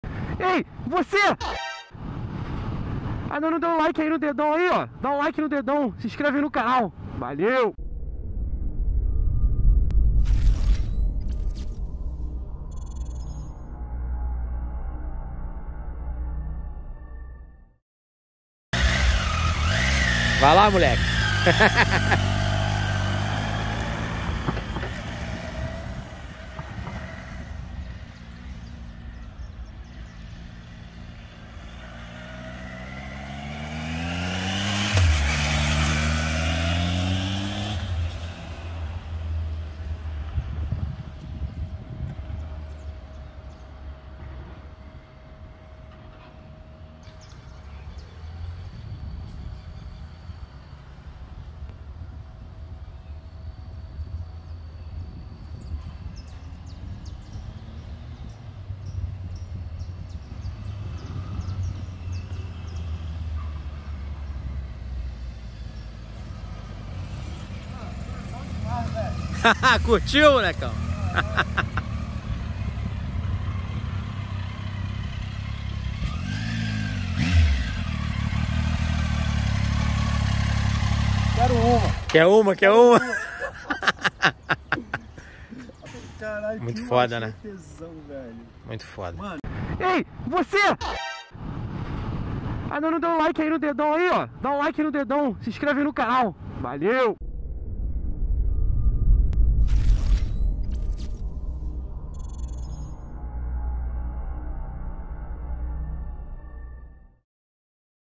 TIGER 800 XCA - CONHECENDO A NOVA MOTOCA DO BRUNO. MUITO TOP.
Fala galera.

Tirando uma casquinha na tiger nova do meu amigo Bruno. Motoca animal demais